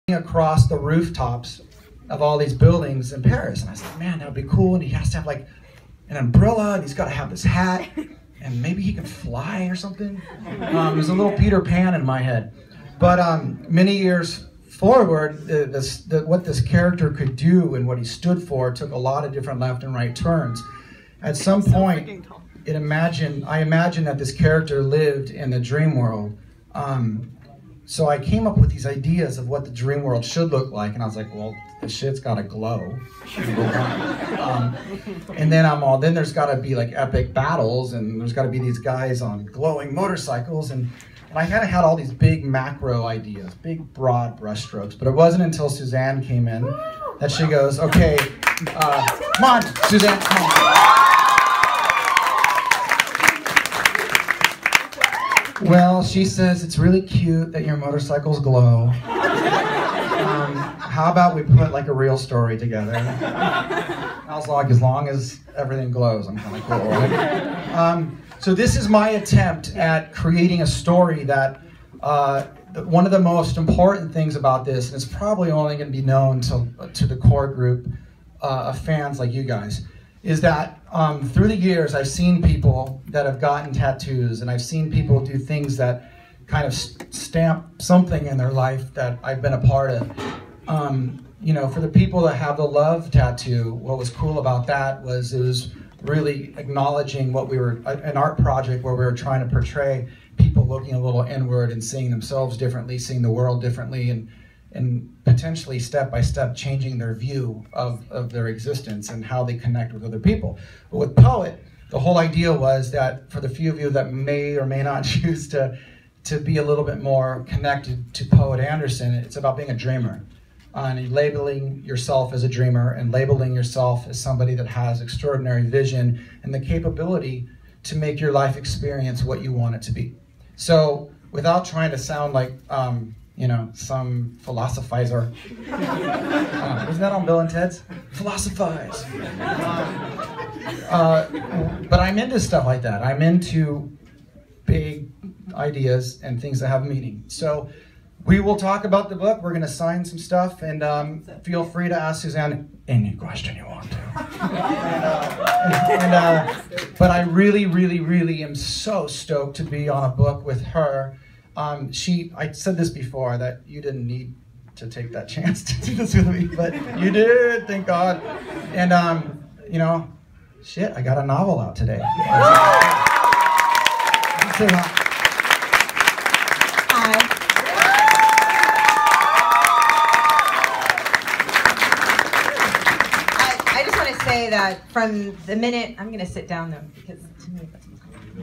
Tom Delonge at Barnes and Noble @ The Grove
Tom Delonge came to the Barnes and Noble at the grove on the day of the release of his new book, "Poet Anderson: Of Nightmares..." along with New York best-selling author, Suzanne Young.